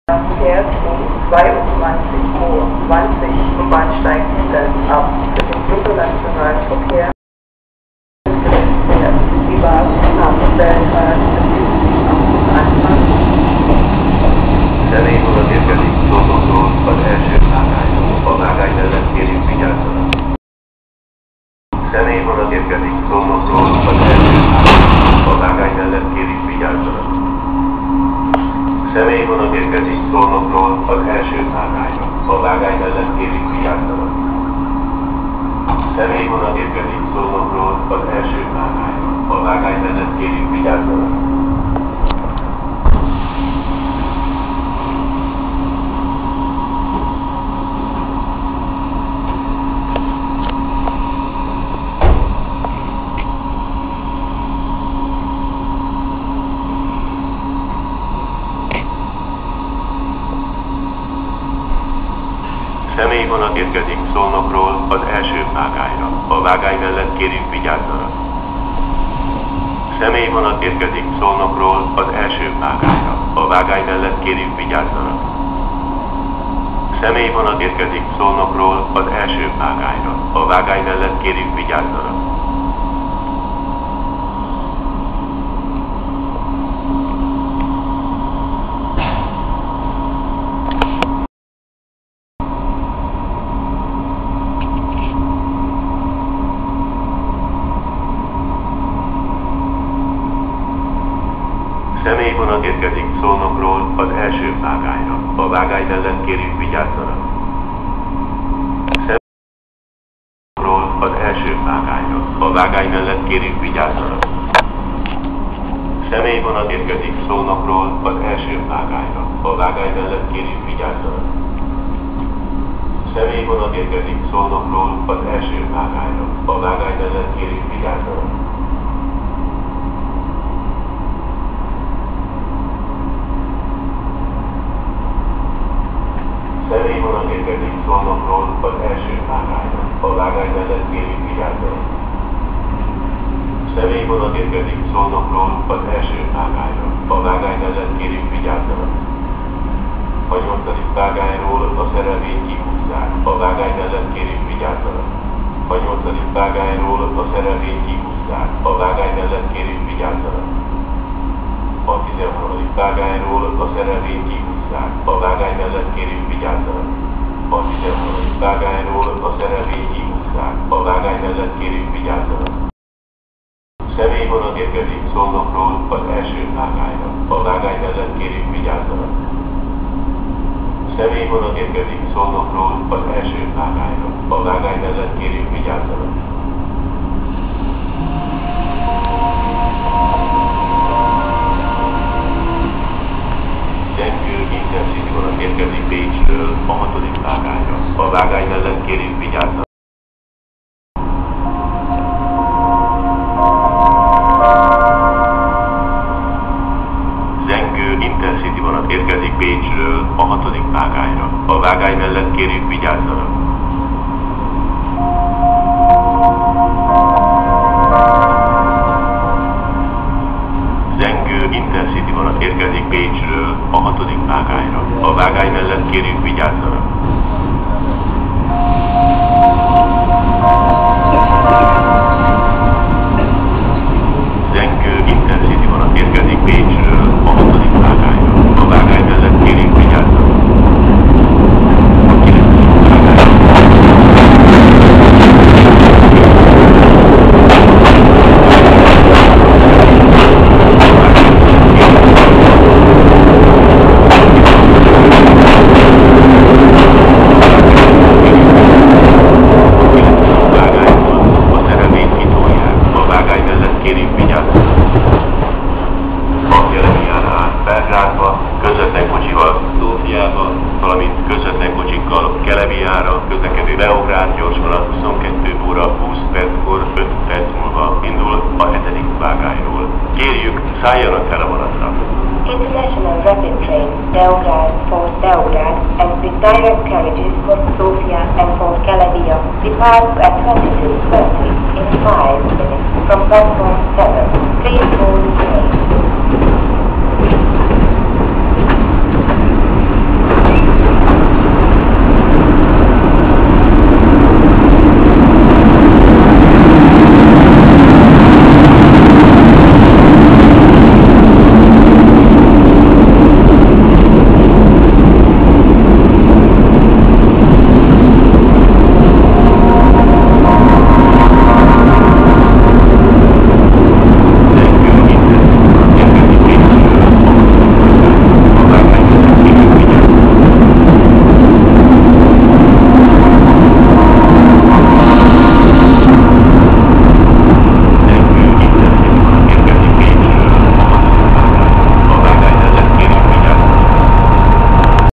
Budapest Keleti 2012.12.30. Dacia, CFR-mozdonnyal, Zengő IC cseh és szlovák kocsikkal
Személyvonat érkezik Szolnokról a 3. vágányra.
Zengő InterCity-vonat érkezik Pécsről a 6. vágányra.
Beograd gyorsvonat indul Belgrádba, közbetlen kocsikkal Szófiába és Kelebiába 22.20 perckor a 7. vágányról., 5 perc múlva
A 9. vágányról a szerelvényt kirolják.